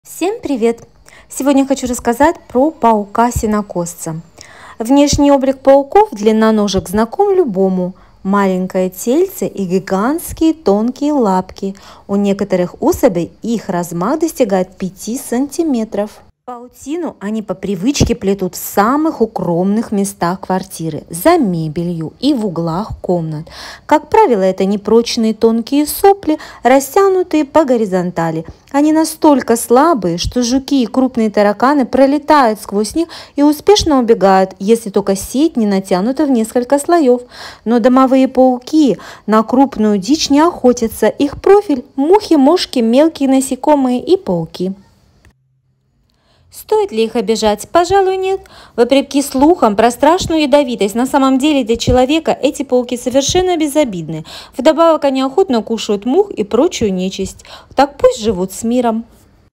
Паук сенокосец. Как выглядит паук сенокосец. Ядовит ли паук-сенокосец?
Ранка сразу же затянется, а вот оторванная лапка ещё несколько минут (до получаса) будет судорожно сокращаться, напоминая движение косы и отвлекая врага. Вот потому‑то животное и получило свои имена – сенокосец, а также простонародные косиножка, косарик, долгоножка.